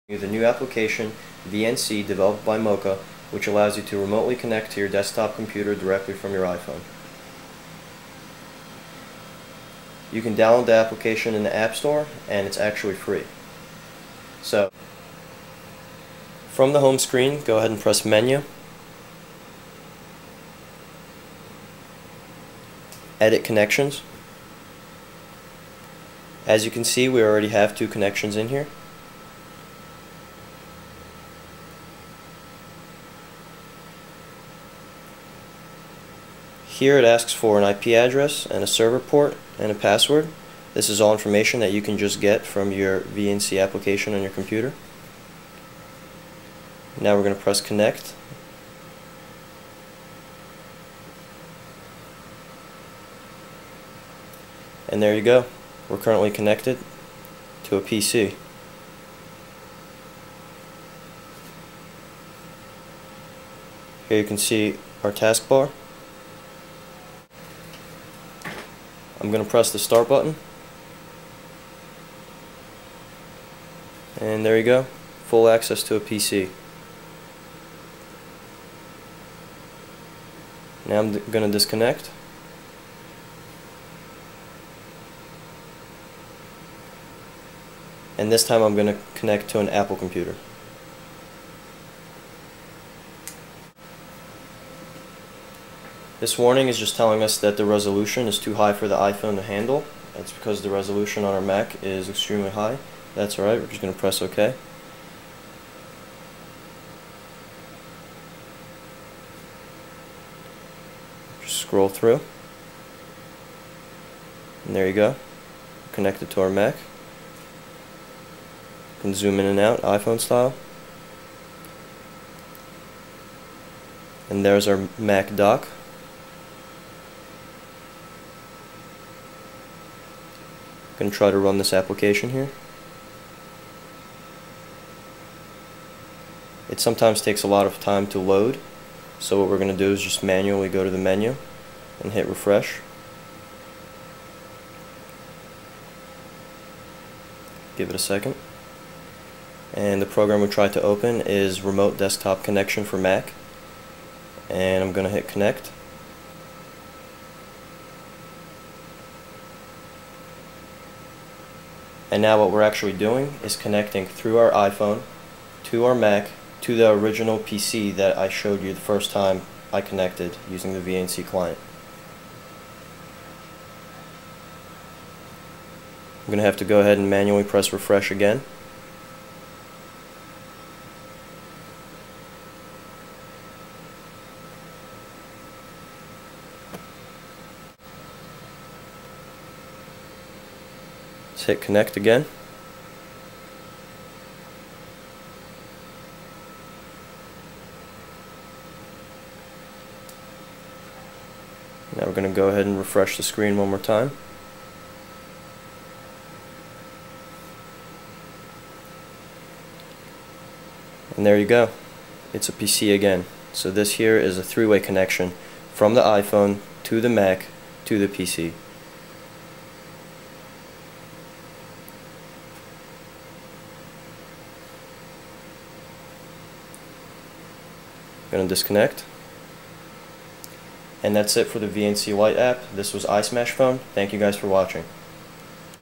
Remotely Control your PC from iPhone via VNC Mocha Lite
Use link above for full article.
A demo of Mocha VNC for the iPhone. Mocha VNC is a free application from the App Store that provides access to a VNC server. Using your iPhone, you can connect to a Windows PC or Mac OS X and see the files, programs, and resources exactly as you would if you were sitting at your desk, just on a smaller screen.